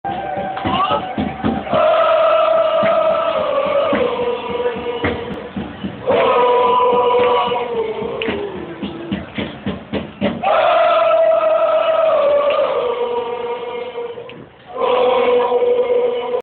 Cove do the chop
cove singing a baseball song? still sounds good